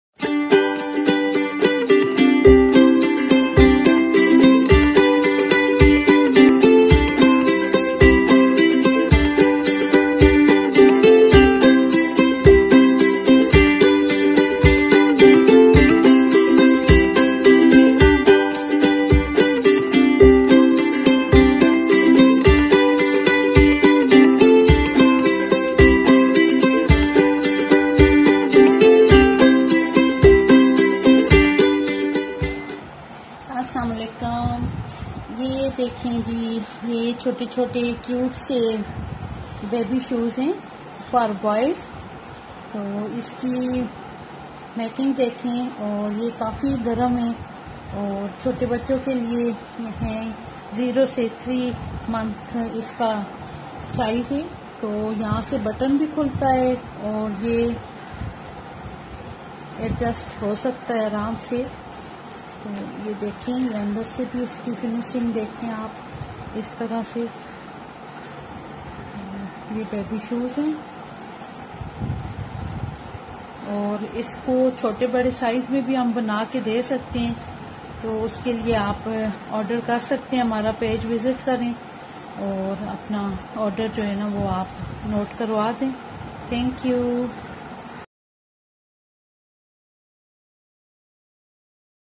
Baby shoes for Boys | Description Video
Hi there..  This is a description video of product made by Amber's Clothing.. You can visit facebook page to order.  This is shoes for 0-3months old boys..  For more visit fb page @ambersclothing77